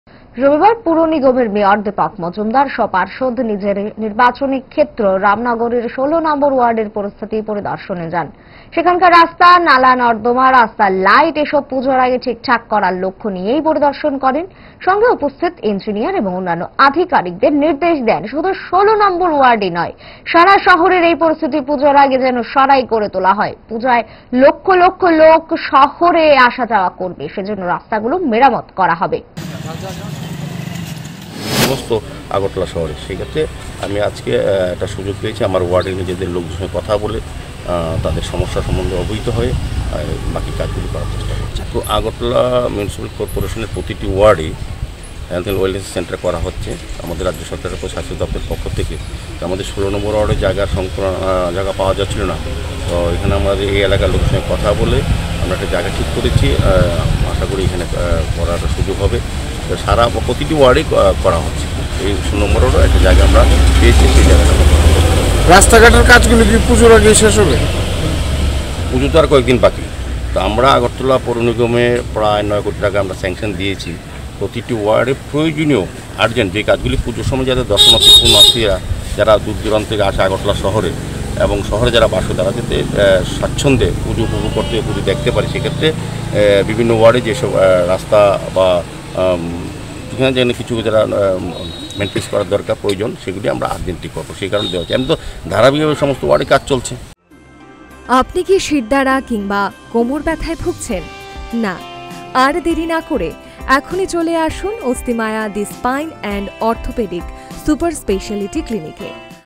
নিজ নির্বাচনী এলাকা পরিদর্শনে গেলেন মেয়র দীপক মজুমদার।
পূজার আগেই পুর নিগমের সব ওয়ার্ডের রাস্তা, নালা নর্দমা সহ রাস্তার লাইট মেরামত করার নির্দেশ দিলেন নিগমের মেয়র দীপক মজুমদার।